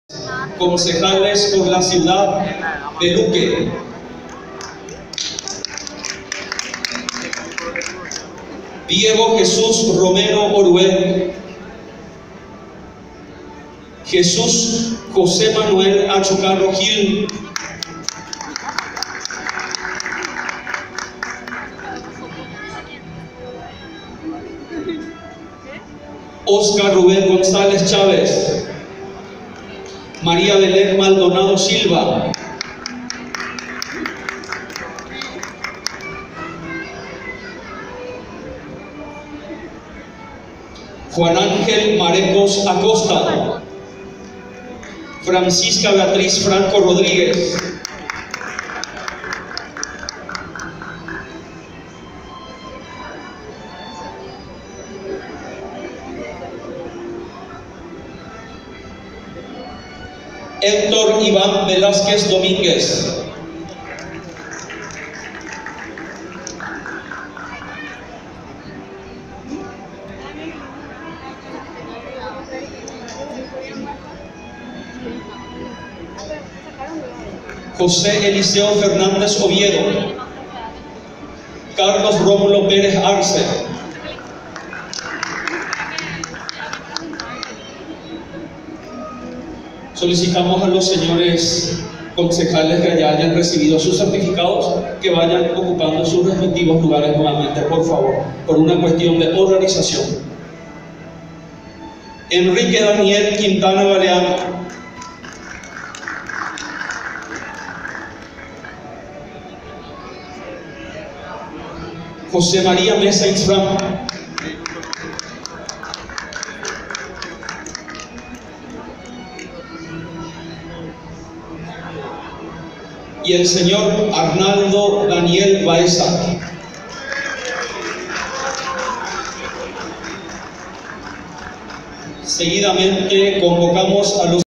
Proclamación de concejales de Luque - Paraguay 2021
Proclamación de autoridades municipales. Anfiteatro "Rafael Rojas Doria" de Villa Elisa, Paraguay. Viernes 5 de noviembre de 2021.
Candidatos electos de Areguá, Guarambaré, Lambaré, Luque, Nueva Italia, Ñemby, San Antonio, Villa Elisa, Villeta, Ypané.